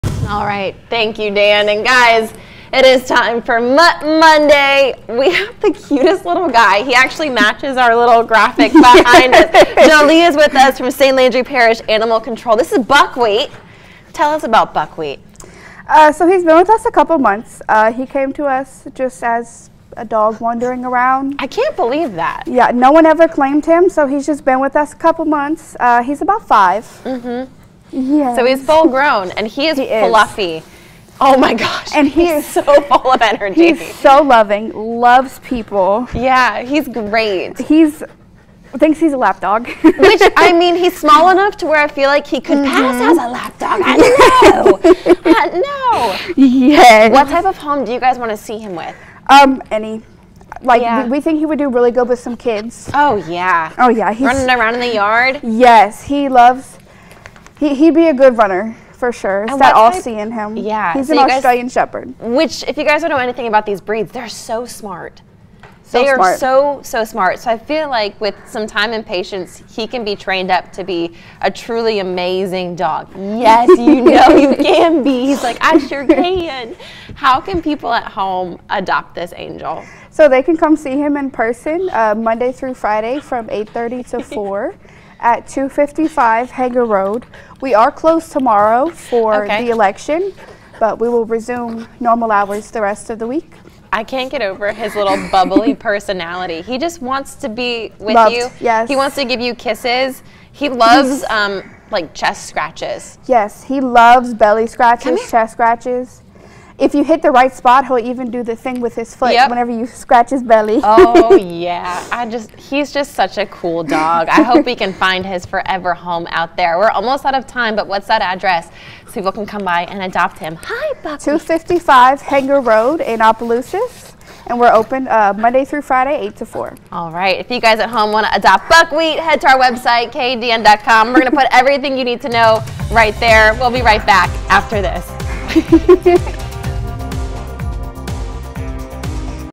Mutt Monday: Adopt Buckwheat! An Energetic Australian Shepard Looking For A Family To Play With
Jolie Rodgers, Animal Adoption Coordinator with St. Landry Parish Animal Control, stopped by First at Four with sweet Buckwheat! Buckwheat is a loveable and energetic Australian Shepard who was found as a stray in St. Landry Parish. He is roughly 5 years old and would thrive in a household with kids or other dogs that he can play with.